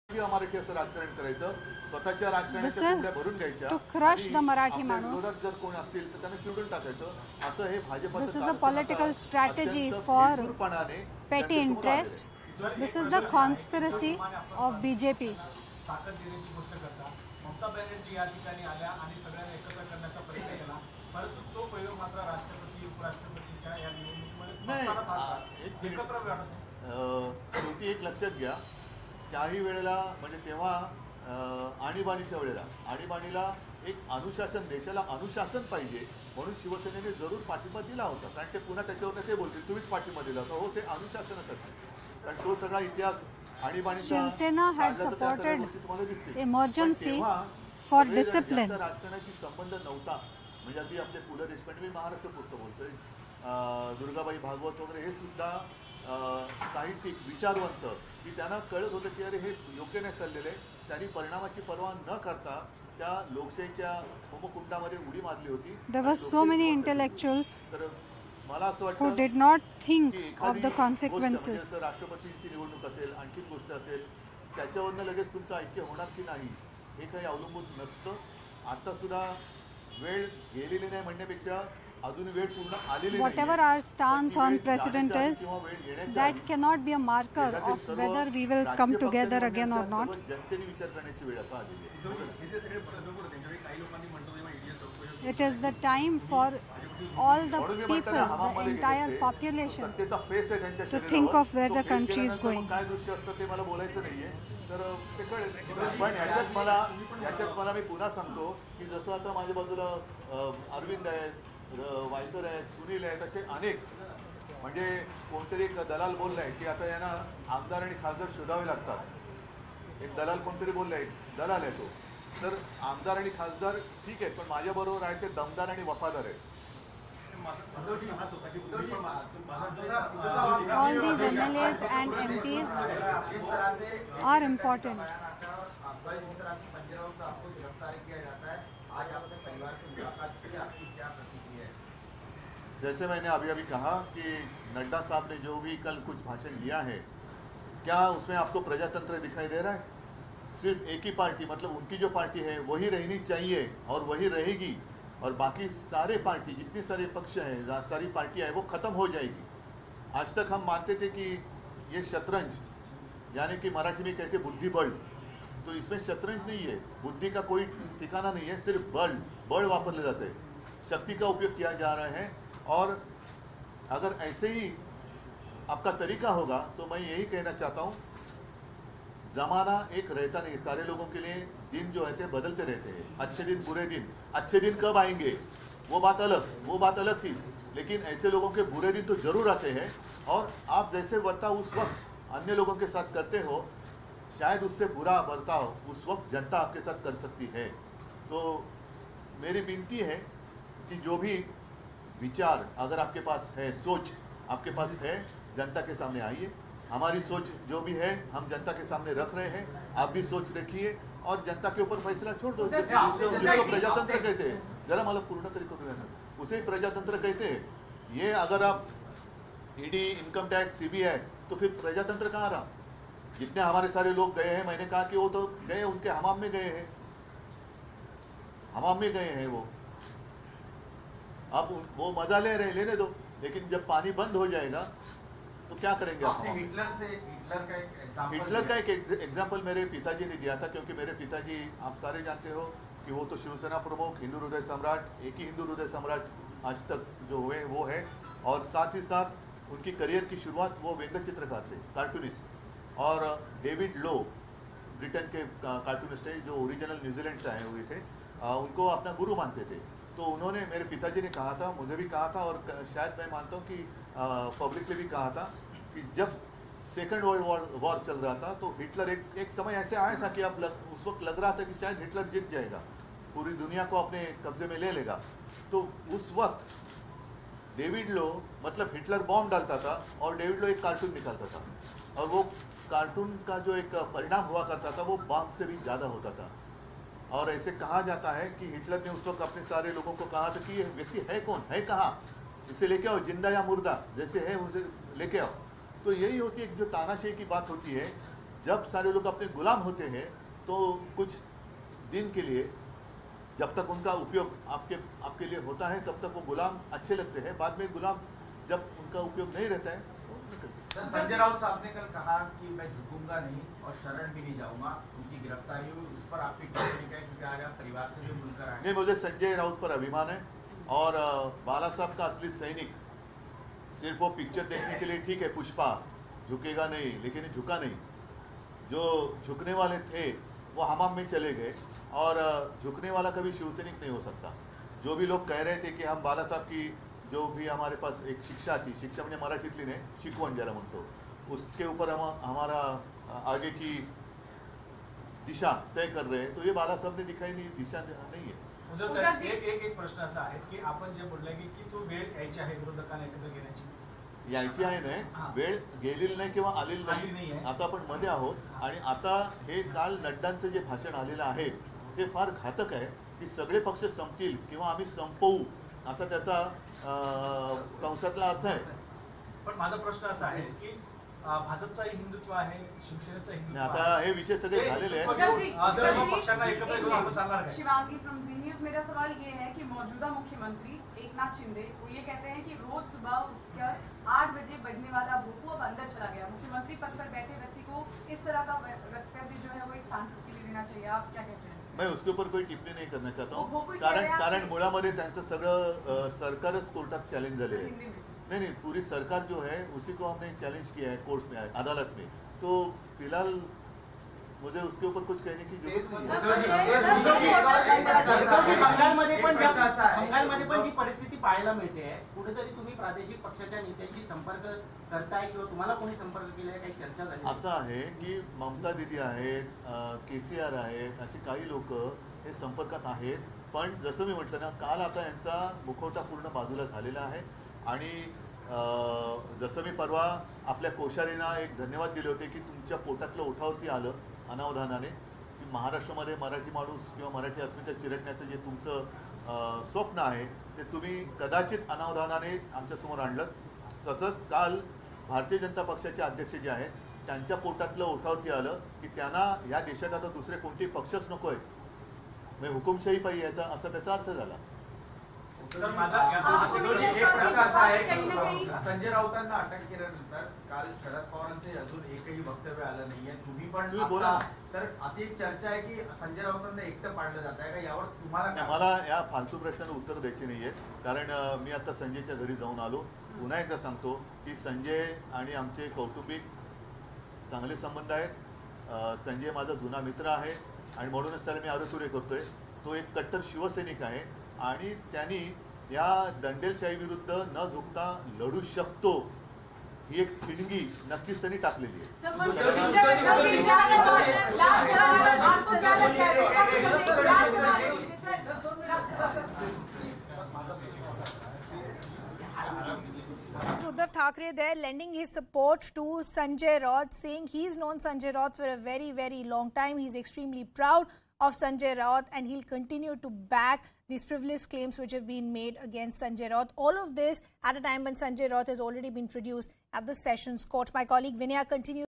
'No One Remains In Power Forever': Uddhav Slams BJP Over Sanjay Raut's Arrest | English News
After meeting with the family members of arrested Shiv Sena MP Sanjay Raut, Shiv Sena Chief Uddhav Thackeray said, "At times, politics is compared with Chess - a game of mind. But now only power and muscle is being used, not brain. But, no one remains in power forever. Times change."